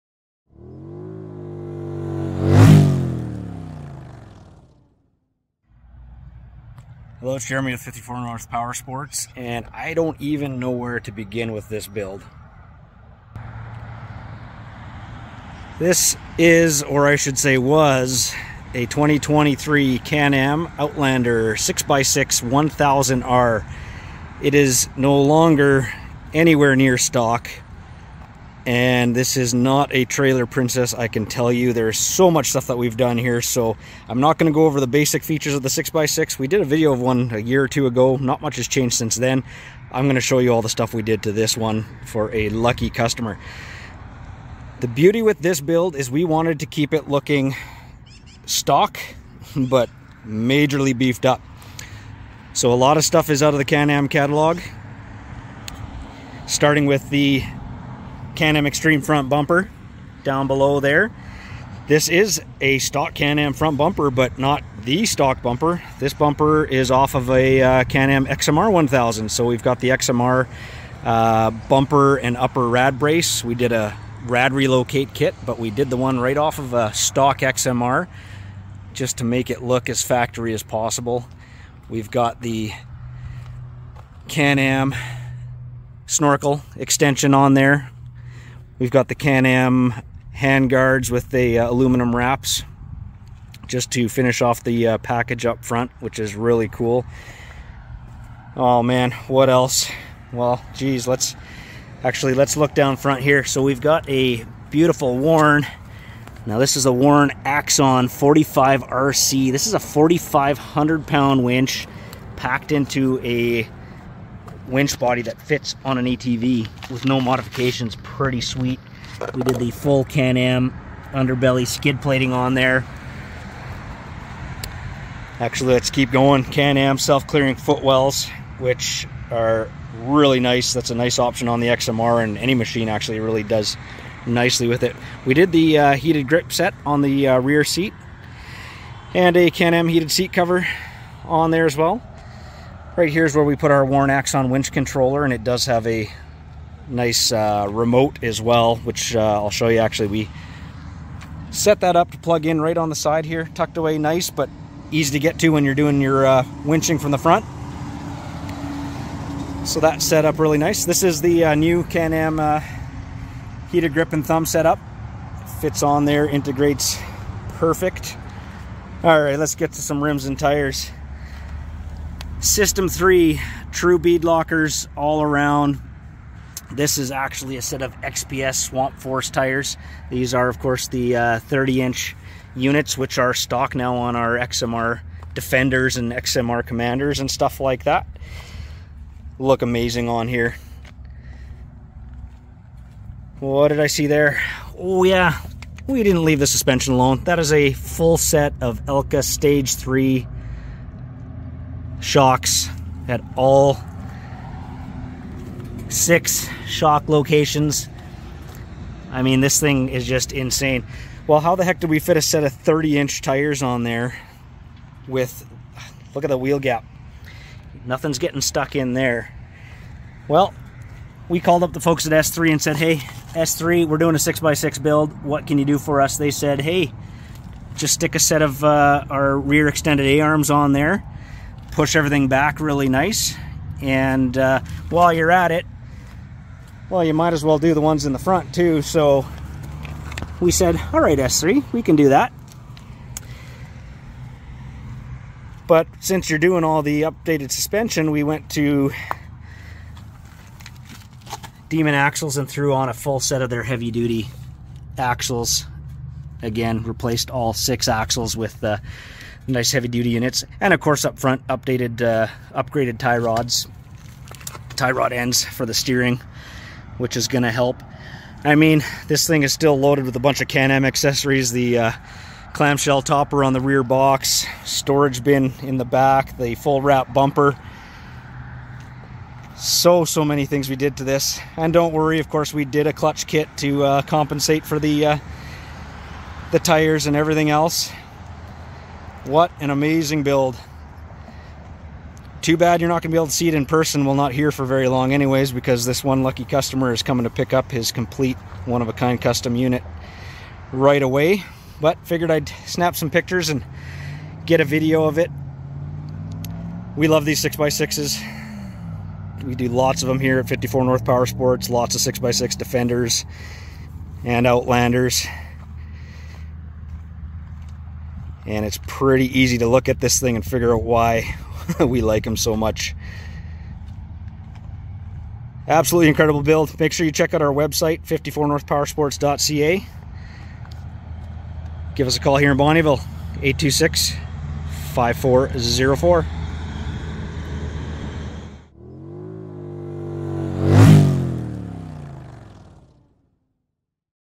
Absolute Unit! Custom Can-Am 6x6 1000 Build.
54 North Powersports Ltd. is an Authorized full-line BRP Dealer.
We are located in Bonnyville, Alberta

👀 Check out this absolute monster build we just completed for a customer!
💪 2023 Can-Am Outlander 6x6 1000
👉 Full Factory Xmr Rad Relocate
👉 S3 Forward Arched A-arms and Trailering arms.
👉 Elka Stage 3 Shock Package
👉 System 3 SB4 Beadlock Wheels
👉 30" XPS Swamp Force Tires
👉 Demon Axles
👉 Warn Axon 45RC Winch